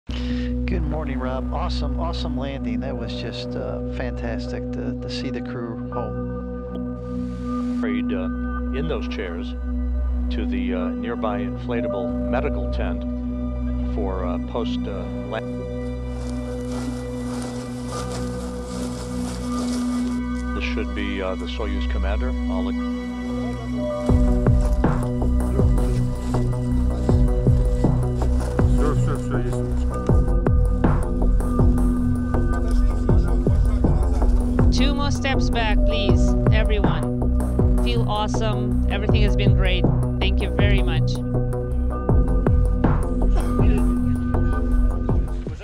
Soyuz Capsule With Crew Of 3 Lands Safely In Kazakhstan To End ISS Mission
A Soyuz MS-24 capsule carrying NASA astronaut Loral O'Hara, Roscosmos cosmonaut Oleg Novitsky, and spaceflight participant Marina Vasilevskaya of Belarus landed safely outside Zhezkazgan, Kazakhstan on April 6.

The astronauts were pulled from the capsule and underwent physical checks after returning from the International Space Station (ISS).

Russian Oleg Novitsky and Belarusian Marina Vasilevskaya departed for the ISS last month on a Soyuz spacecraft, while U.S. astronaut Loral O'Hara had been aboard the orbital station since September.

n18oc_world      n18oc_crux